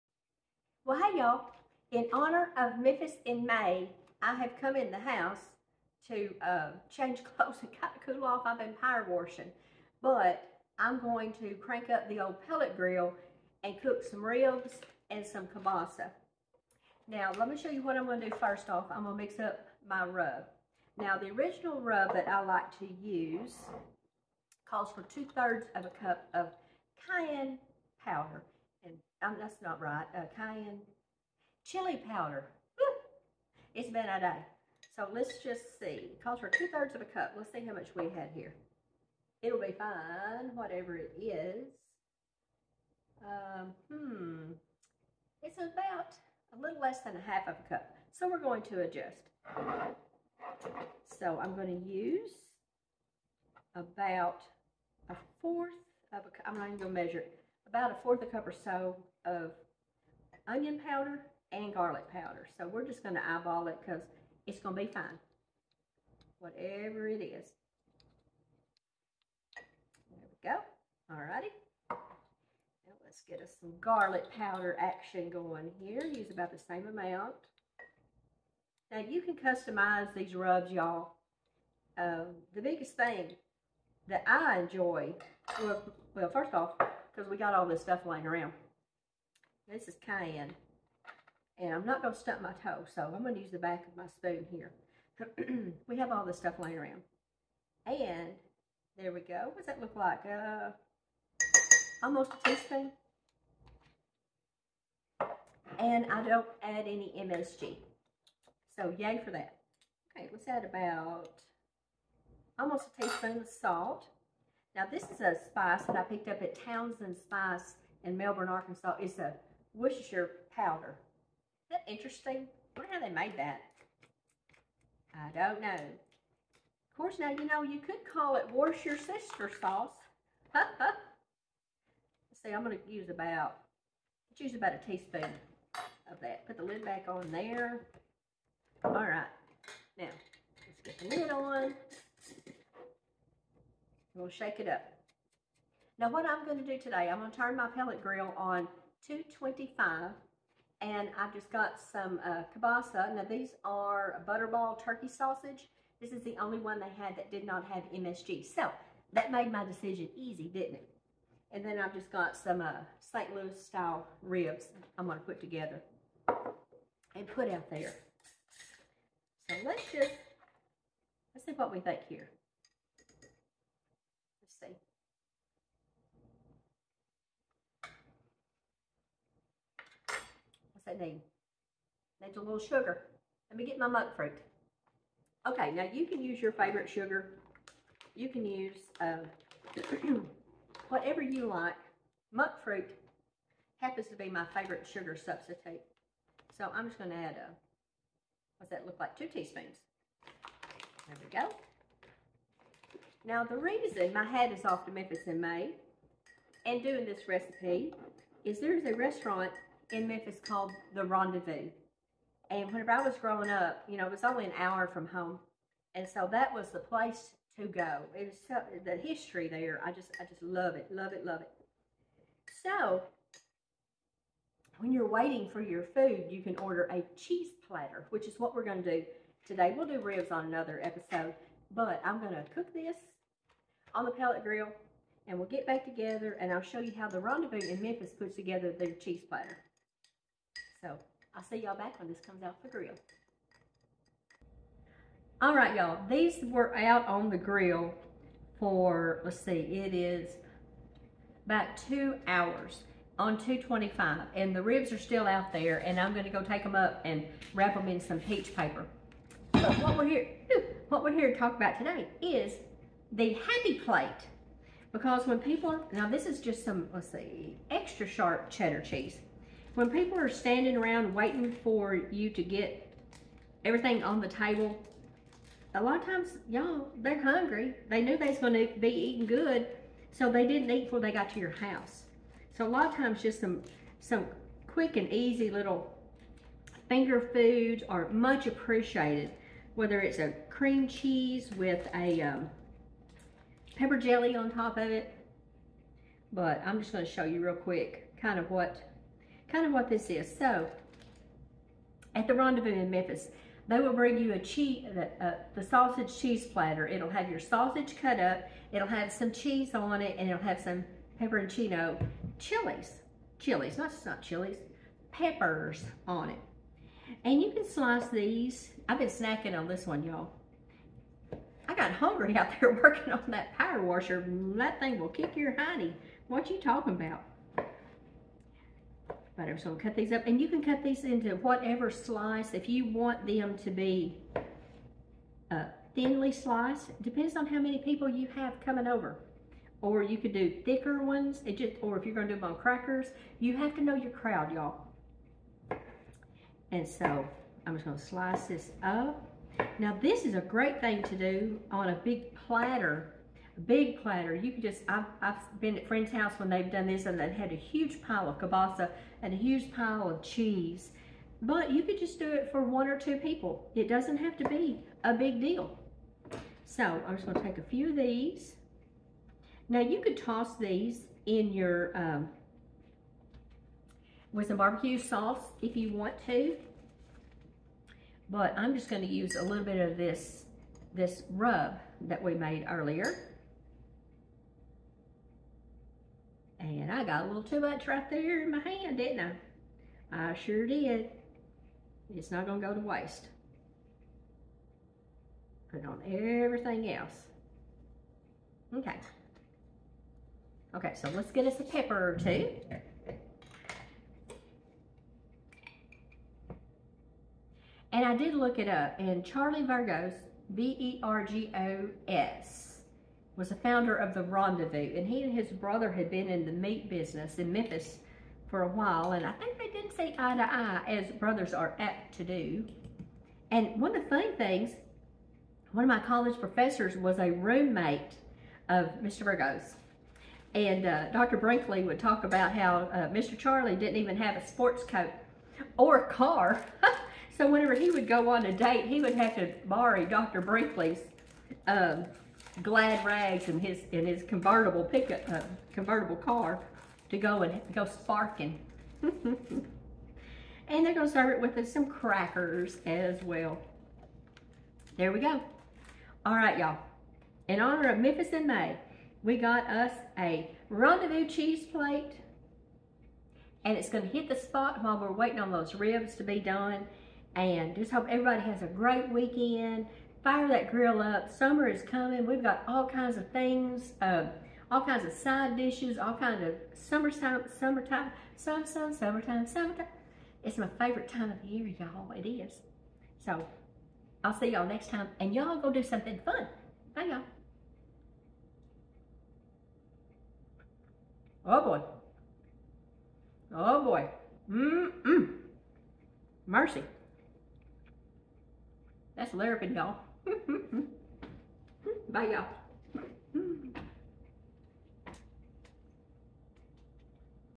Smoked Kielbasa in honor of MemphisInMay
Smoked Kielbasa for a Memphis In May, Rendezvous Restaurant inspired sausage and cheese platter.

bbq rub
2/3 c chili powder
2 T sugar or sugar substitute
1/4 c onion powder
1/4 c garlic powder
1 tsp cayenne pepper
1 tsp Worcestershire sauce powder